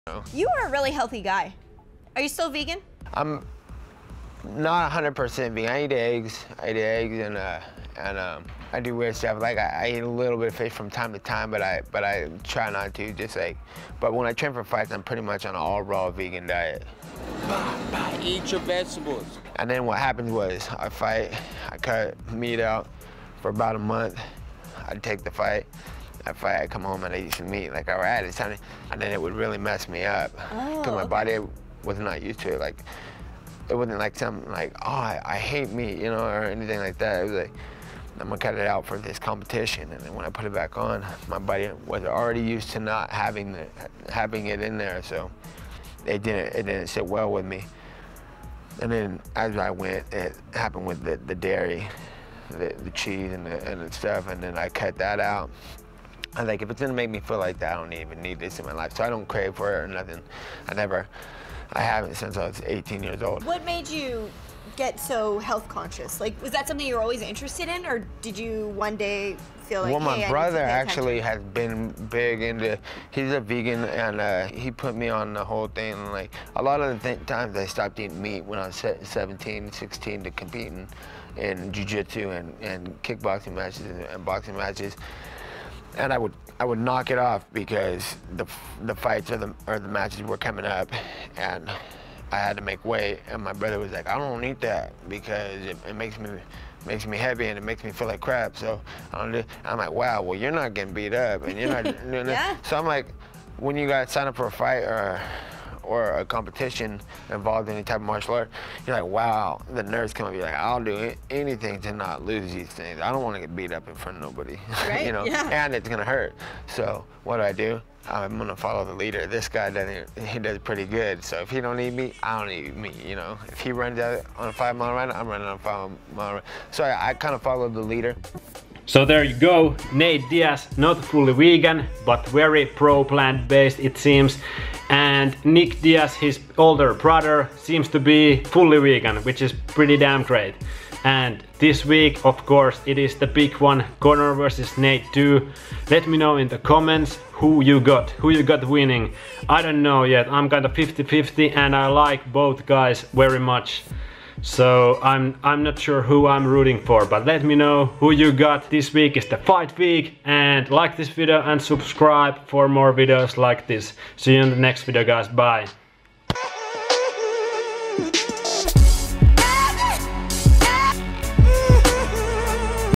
Nate Diaz Talks About Vegan Diet Before UFC 202
Nate Diaz talks about veganism, vegan diet, healthy eating and his brother Nick Diaz to Megan Olivia before his rematch against Conor McGregor at UFC 202